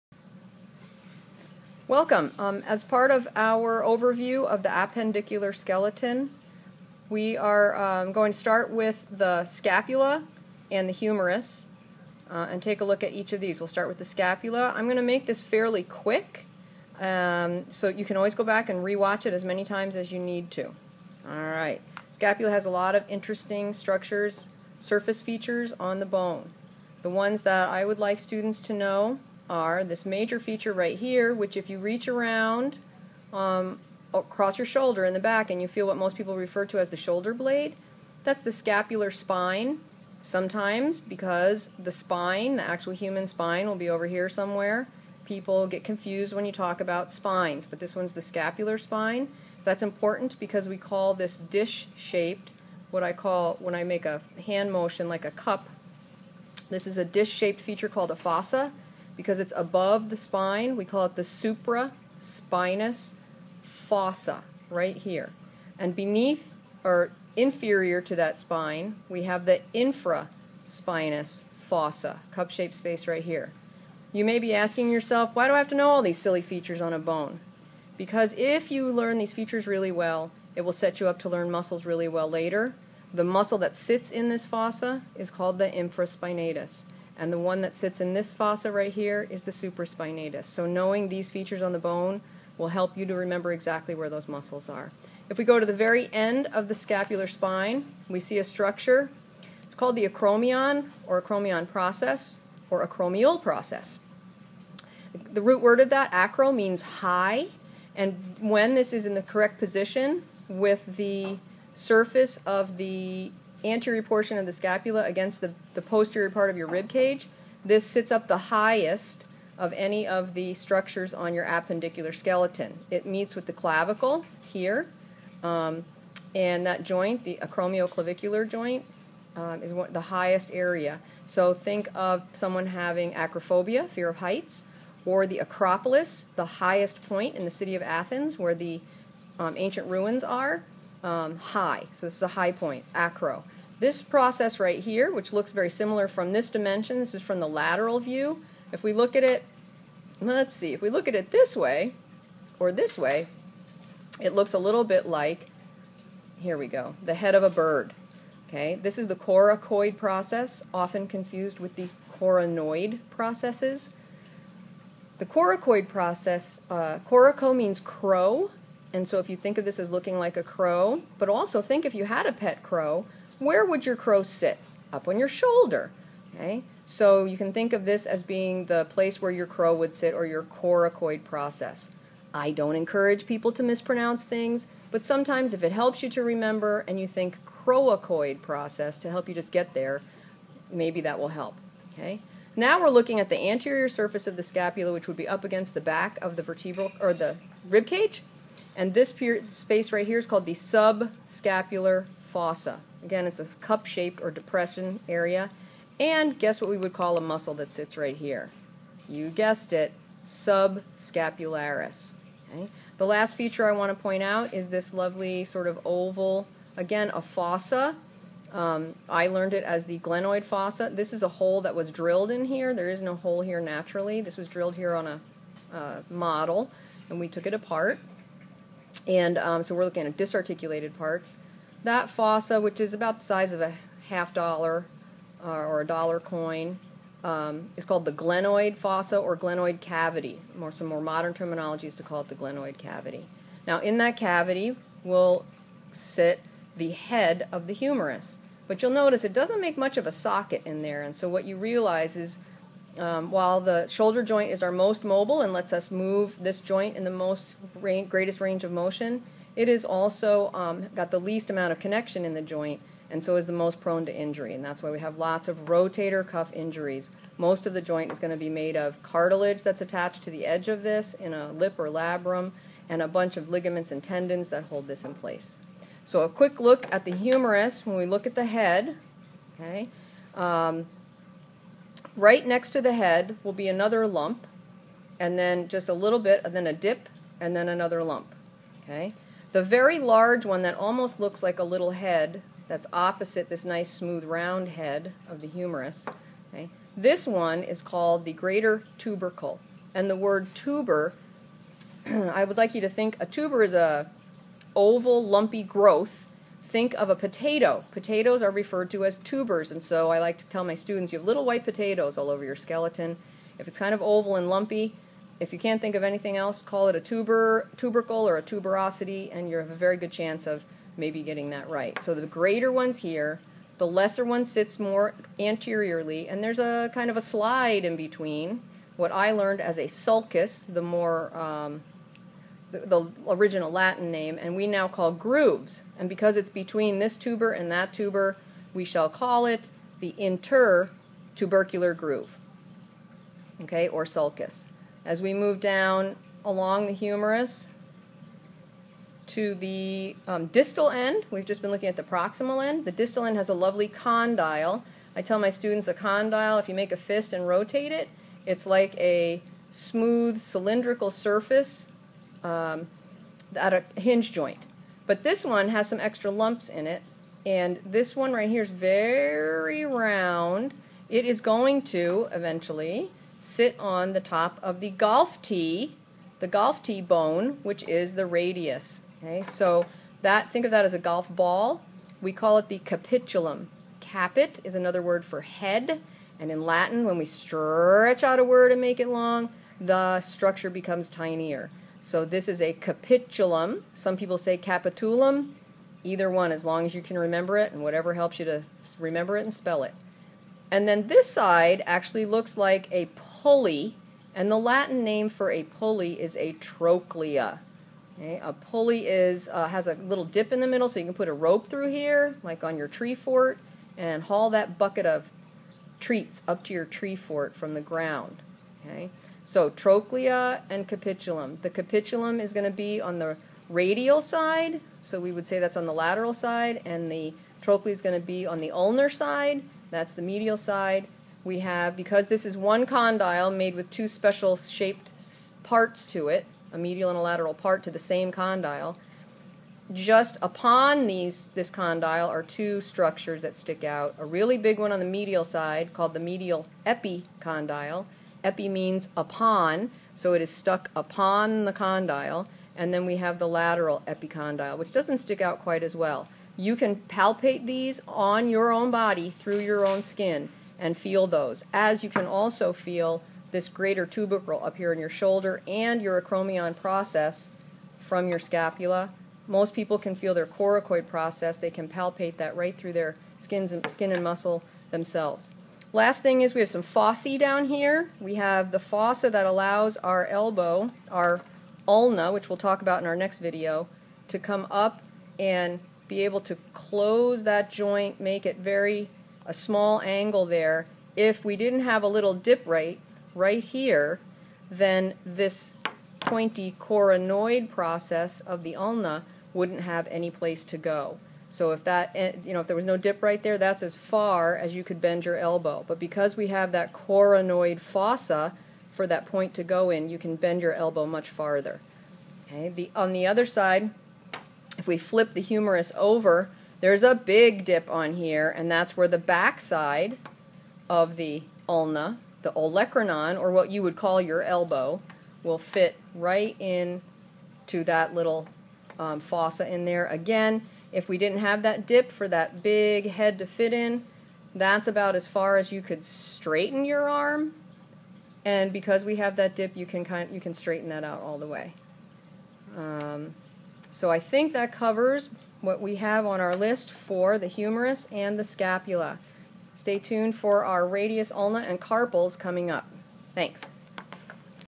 Prof. Wilson on the scapula and humerus The best Teacher!!
Prof. Wilson covers the Appendicular Skeleton, this video is the bones of the scapula and humerus. This is a must see for A&P students.